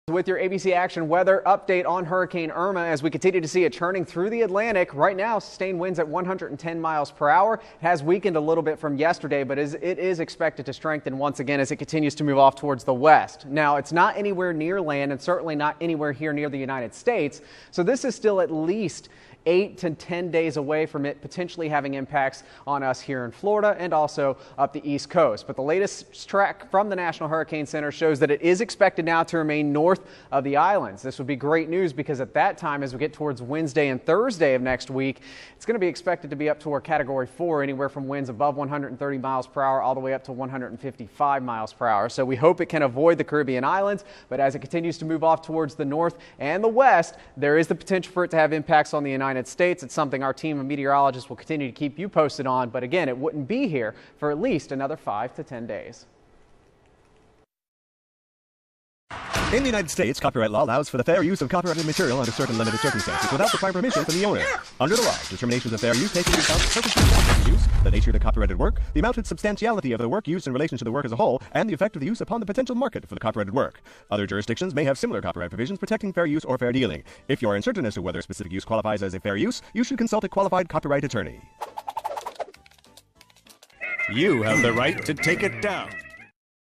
Hurricane Irma Forecast (Florida)
Hurricane Irma, the next 'Big nasty' is on track to make landfall Florida in about a week's time. After watching Hurricane Harvey my advice to locals is to buy a small boat.